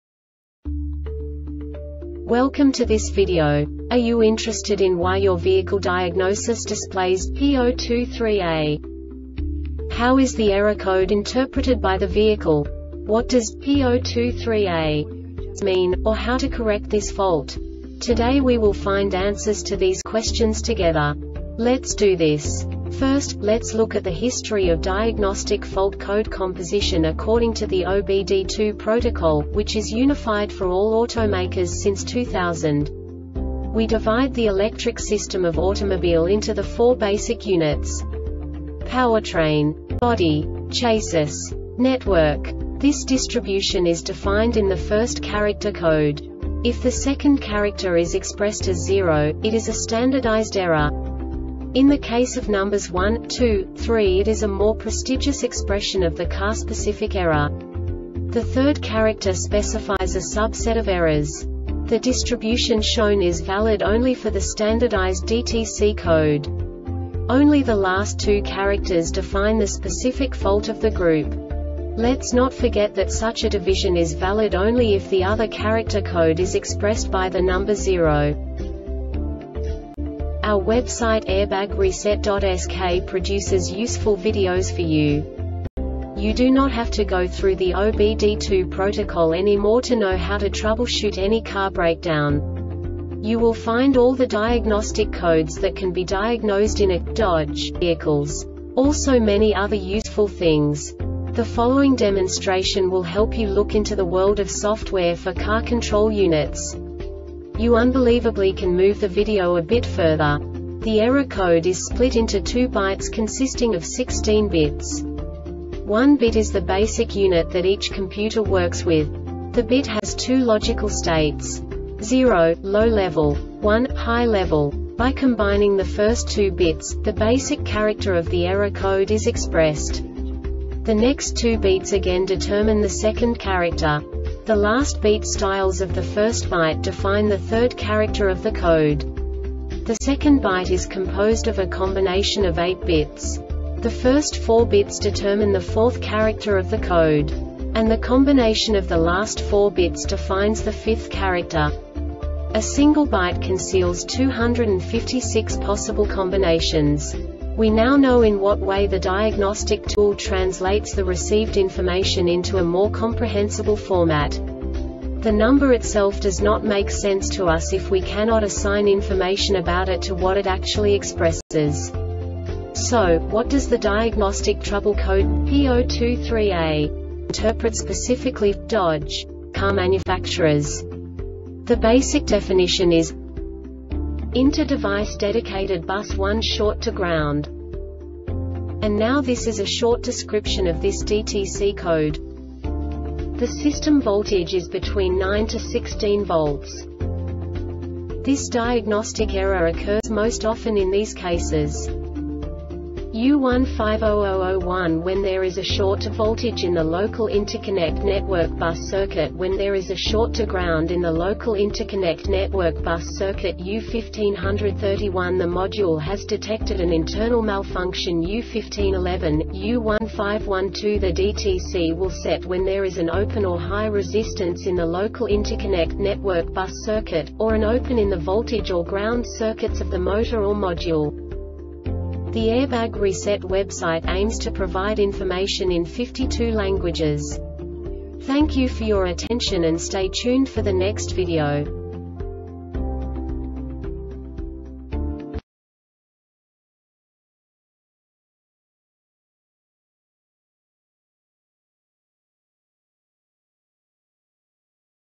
DTC Dodge P023A Short Explanation
Contents:
0:21 Basic DTC analysis according to OBD2 protocol standard.
1:48 Insight into programming
2:58 A diagnostically understandable description of the Diagnostic Trouble Code
3:05 Basic error definition

Make:
Dodge
Code:
P023A
Definition:
Inter-Device Dedicated Bus 1 Short to Ground
Description:
The system voltage is between 9-16 V.
Cause:
U1500 01When there is a short to voltage in the local interconnect network bus circuit.When there is a short to ground in the local interconnect network bus circuit.U1500 31The module has detected an internal malfunction.U1511, U1512The DTC will set when there is an open or high resistance in the local interconnect network bus circuit,or an open in the voltage or ground circuits of the motor or module.